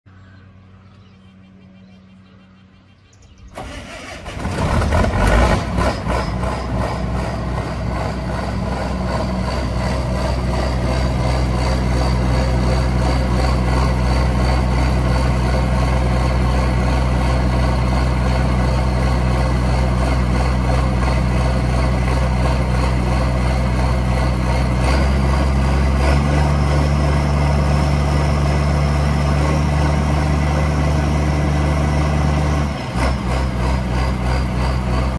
1997 NEW HOLLAND TR97 TWIN ROTOR COMBINE 20
1997 New Holland TR97 Twin Rotor Combine, AWD, 18.4/15-26 steering, 30.5L-32 drive tires, 3,394 machine hours, Serial # TR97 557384